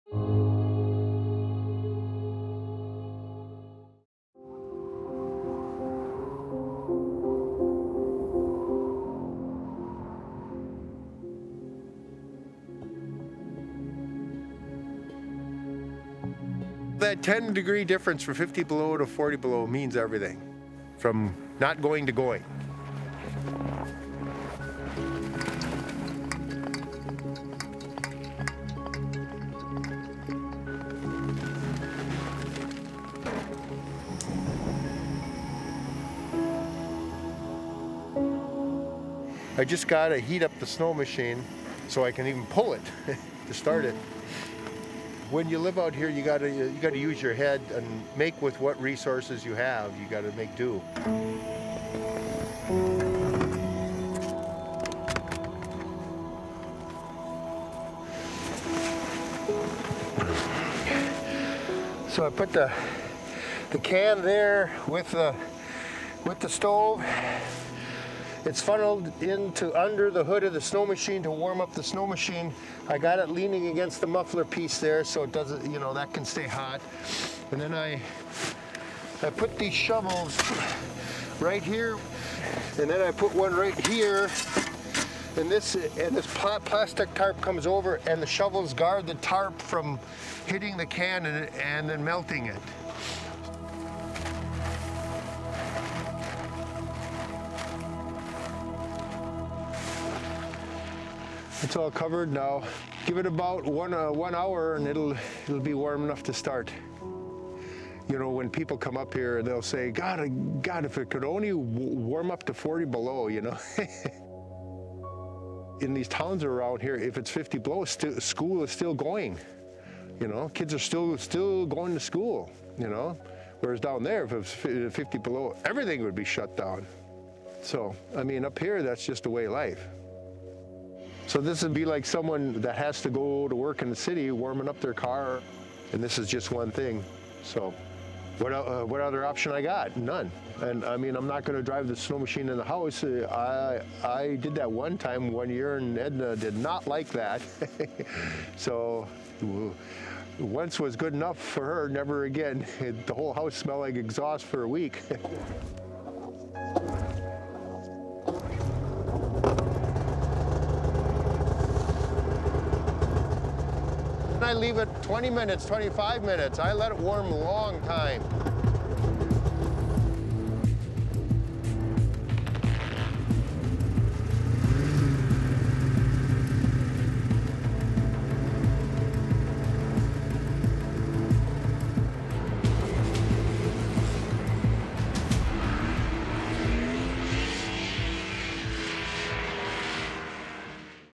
When It's 40 Below and Your Snowmobile Won't Start
You've got to use your head in the wilderness. When it's 40 below and your snowmobile won't start, this is how you warm it up.